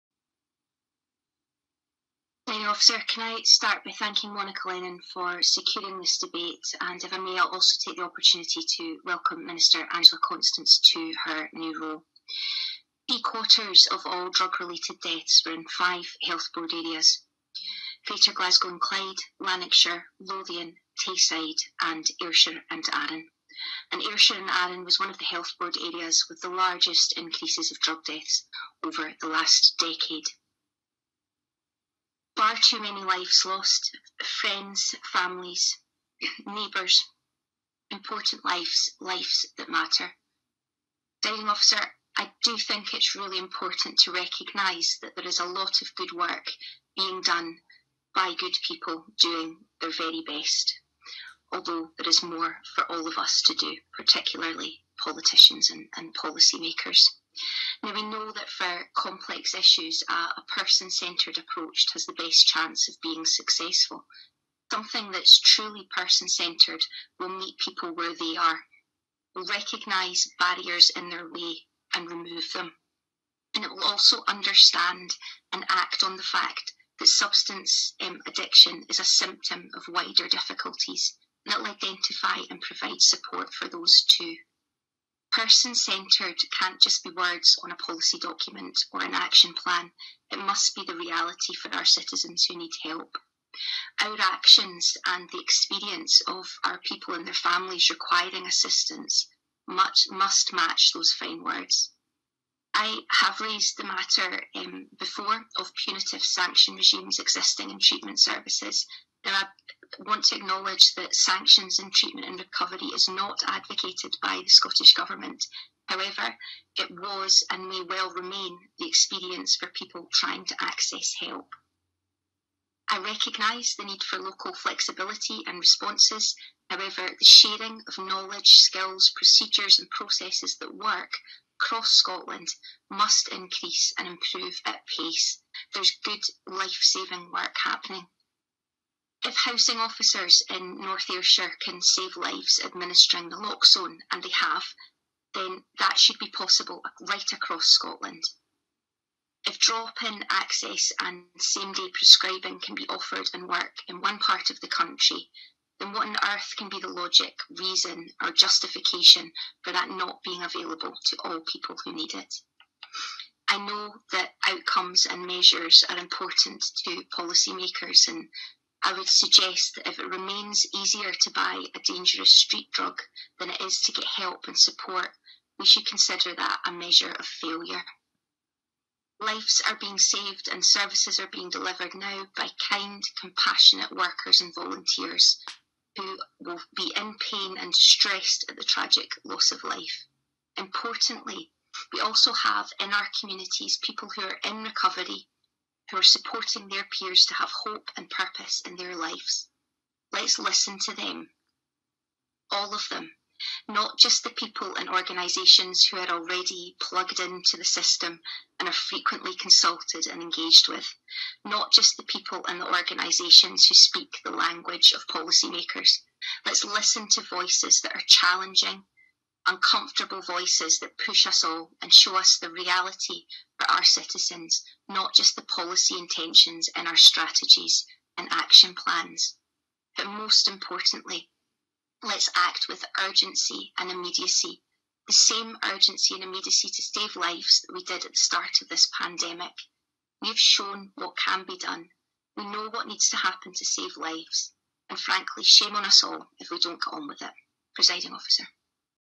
Scotland's Drug Deaths Crisis
Ruth Maguire MSP's contribution to the Scottish Parliament's Member's Debate on Scotland's Drugs Death's Crisis on 14/01/2021.